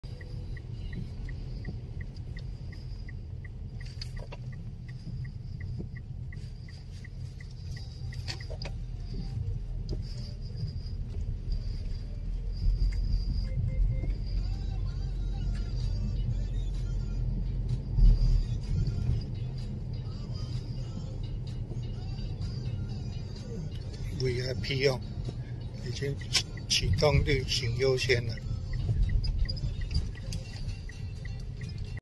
Ambulance
消防優先綠行是萬物聯網公司六年前2017完成的創新功能，利用訊號指標，我們的無線號誌偵測救火車、救護車的距離與方向，正確判斷他是要通過哪一個路口及參考哪一組紅綠燈，然後我們在救護車到達路口前150-200公尺，我們系統就啟動減秒換綠功能，讓救火車救護車百分之百一釘在綠燈情況下安全通過路口。
無論是紅黃綠燈，救護車到達路口時，駕駛員不必做任何操作，只需專心開車，控制器自然會為，他換成綠燈，等他安全通過。